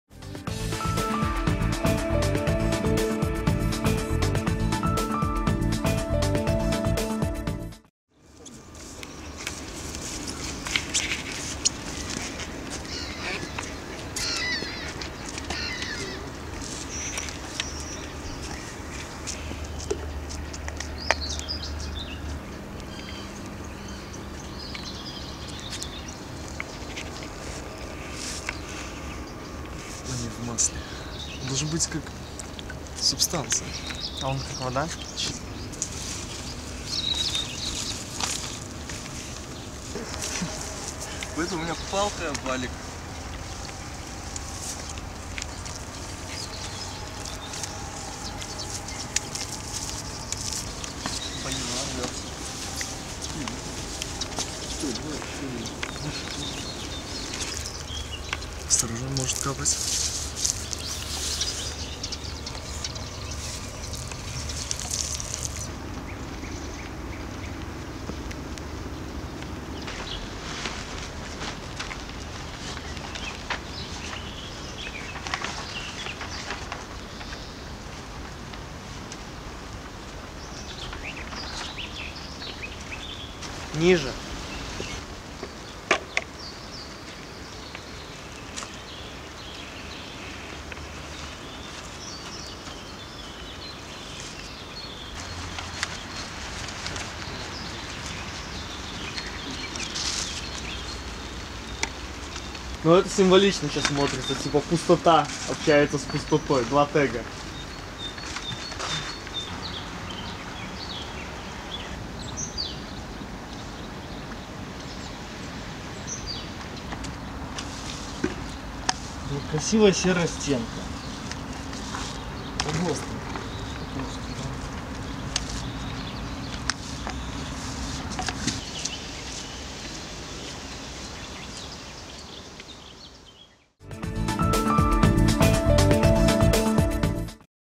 Вулічны мастак ноччу размаляваў сцены ля КЗ "Мінск"
Вулічны мастак Максім Самасевіч, вядомы як Продак, упрыгожыў шэрыя сцены ў парку бліз КЗ "Мінск" арыгінальнымі постэрамі. Дапамагаў яму з гэтым каардынатар праекта "Сігнал" Алег Ларычаў. Хлопцы пачалі працу а палове на пятую раніцы, скончылі праз паўтары гадзіны. Як гэты было, глядзіце на відэа.
Больш працаў беларускіх вулічных мастакоў можна пабачыць на выставе ў Музеі сучаснага выяўленчага мастацтва (г.Мінск, пр-т Незалежнасці, 47).